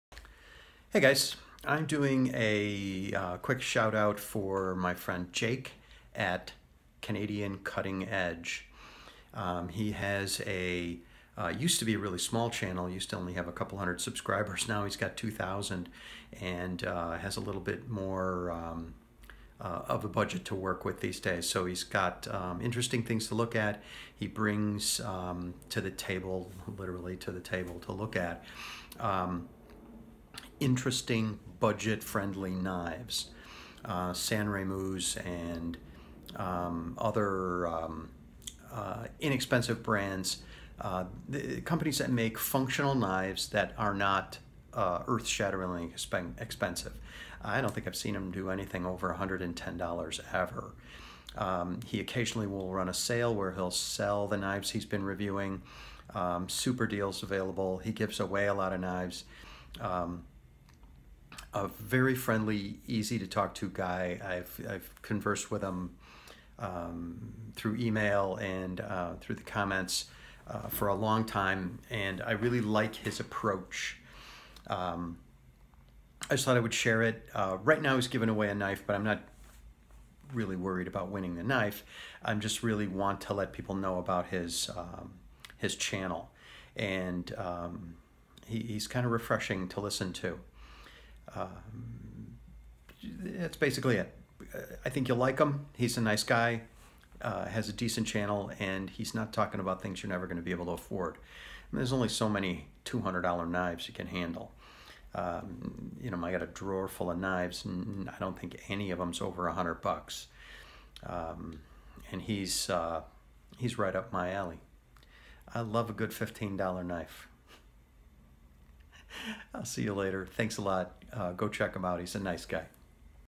Shoutout for Jake
Jake at Canadian Cutting Edge reviews budget friendly knives that you may have missed.